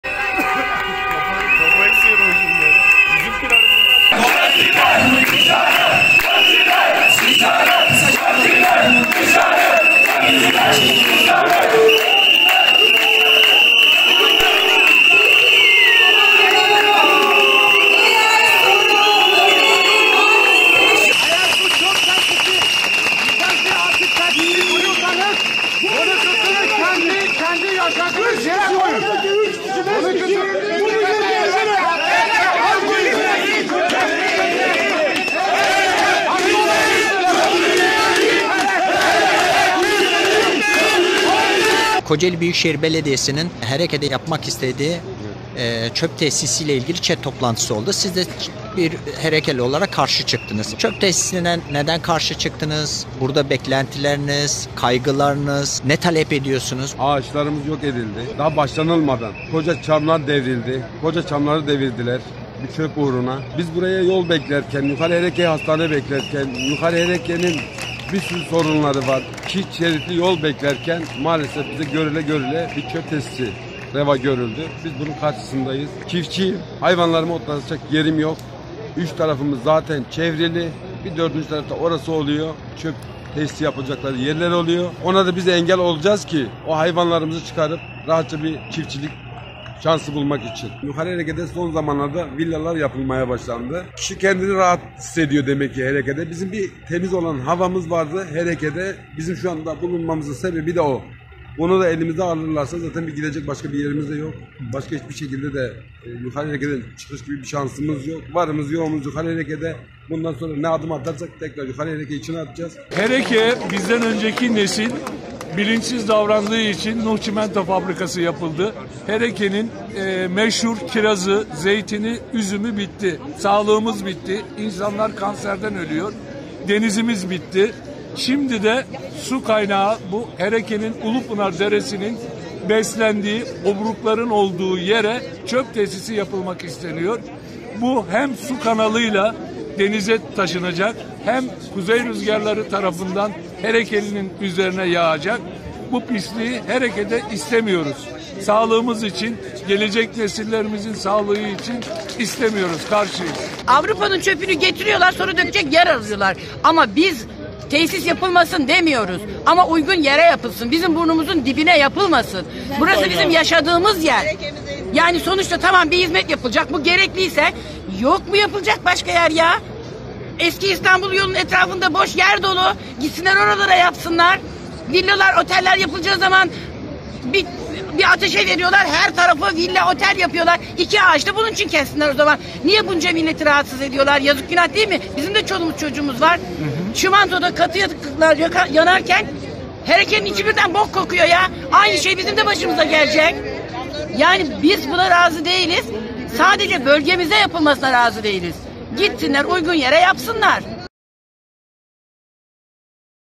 Hereke halkı çöp tesisi projesinin ÇED toplantısını protesto etti: "Hereke kimsenin çöplüğü değil!"
Kocaeli Büyükşehir Belediyesi tarafından Körfez Yukarı Hereke’de 328 dönümlük arazi üzerine yapılması planlanan çöp tesisi için gerçekleştirilen ÇED toplantısına yöre dernekleri, mahalle muhtarları ve çöp tesisine karşı olan Körfez halkı katıldı.

Prof. Dr. Ahmet Haluk Dursun Hereke Kültür Merkezi’nde yapılmak istenen ÇED toplantısını protesto eden vatandaşlar “Avrupa’nın çöpünü buraya getirecekler” “Hereke kimsenin çöplüğü olmayacak” diyerek tepkilerini dile getirdi.